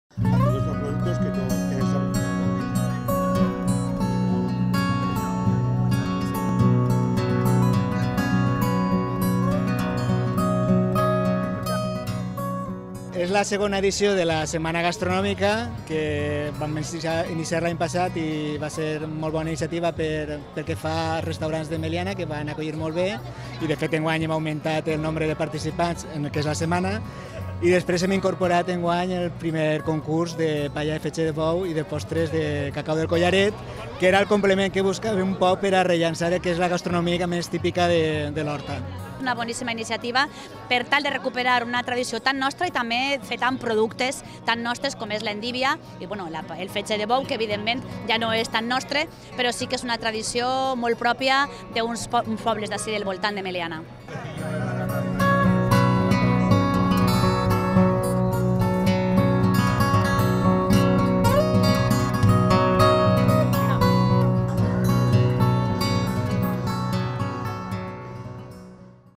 El I Concurs de paella de fetge de bou de Meliana reuneix una vintena de restaurants
La vicepresidenta de la Diputació, Maria Josep Amigó, ha inaugurat el I Concurs professional de paella de fetge de bou i postres del cacau del collaret, emmarcat dintre de la II Setmana Gastronòmica de Meliana ‘Menja’t Meliana’, i que ahir va concloure amb gran afluència de visitants, amb un total de 20 restaurants de tot el territori valencià.